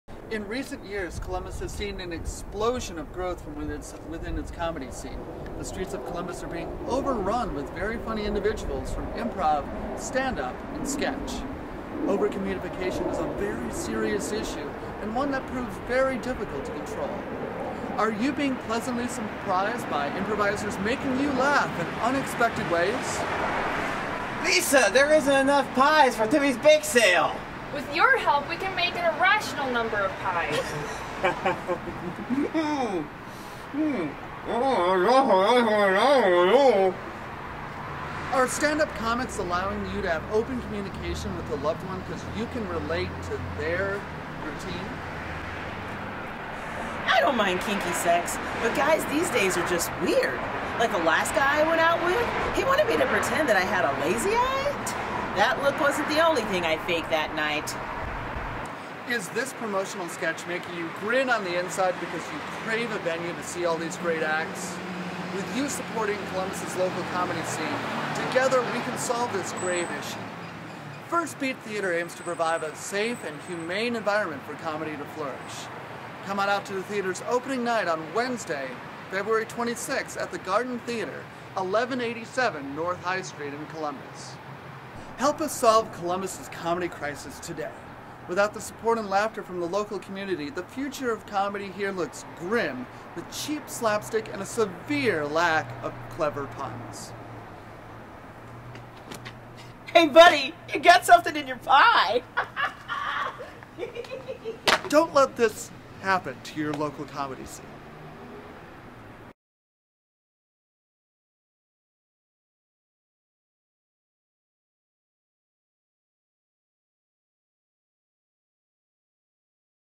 First Beat Theater Opening Night Promo
Come out to First Beat Theater's opening night
Wednesday February 26th at 8pm

Held at the Garden Theater
1187 North High St.